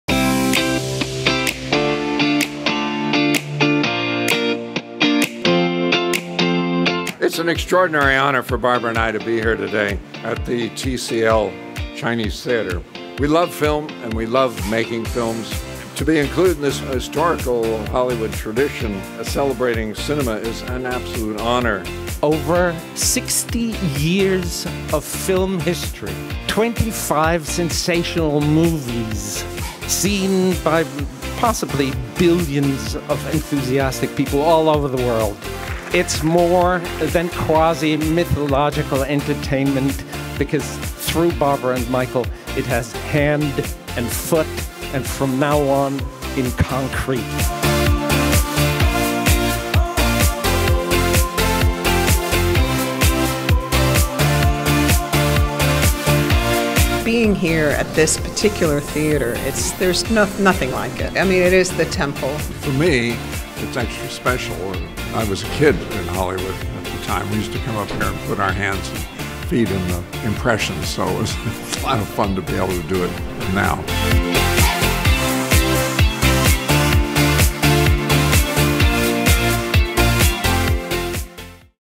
Michael G. Wilson & Barbara Broccoli Imprint Ceremony - 60th Anniversary of James Bond
Watch Michael G. Wilson and Barbara Broccoli put their imprints in cement to celebrate 60 years of James Bond!